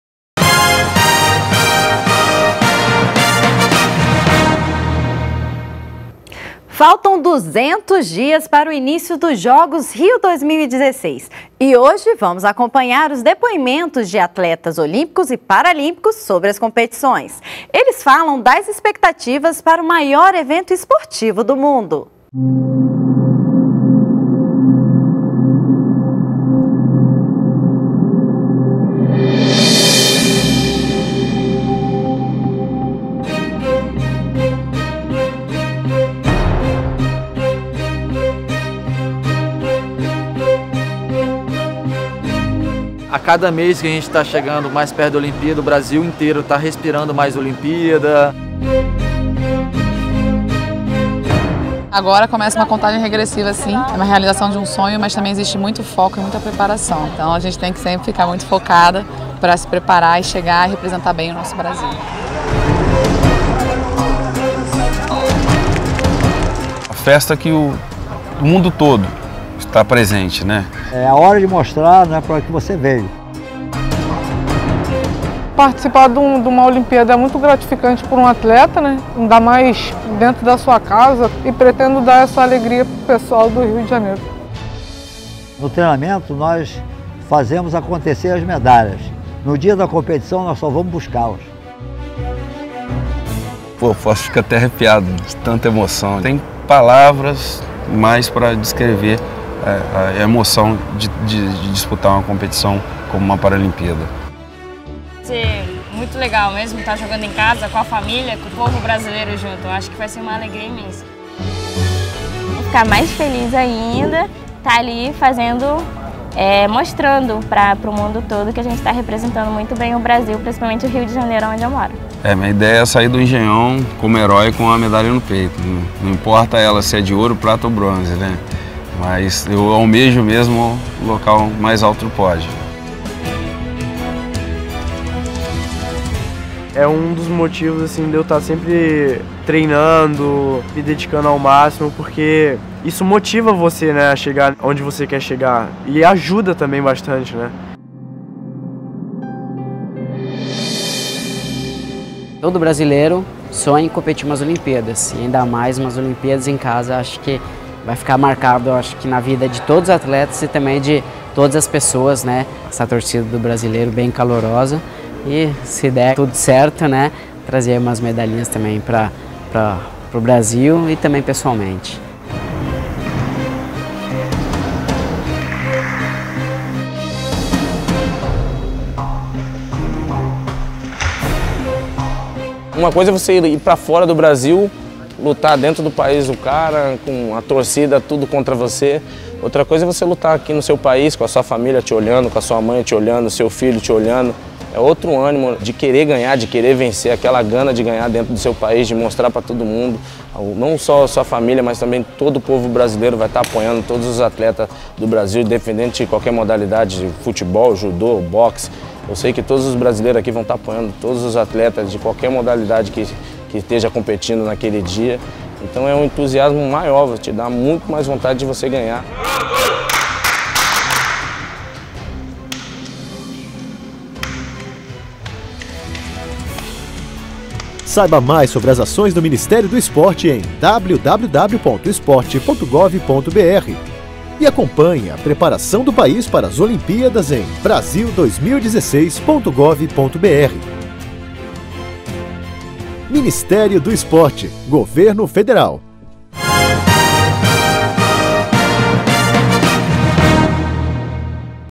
Atletas falam sobre a expectativa para os Jogos Olímpicos do Rio
BRASIL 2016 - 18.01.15: Faltam 200 dias para o início dos Jogos Rio 2016. Vamos acompanhar os depoimentos de atletas olímpicos e paralímpicos sobre as competições. Eles falam das expectativas para o maior evento esportivo do mundo.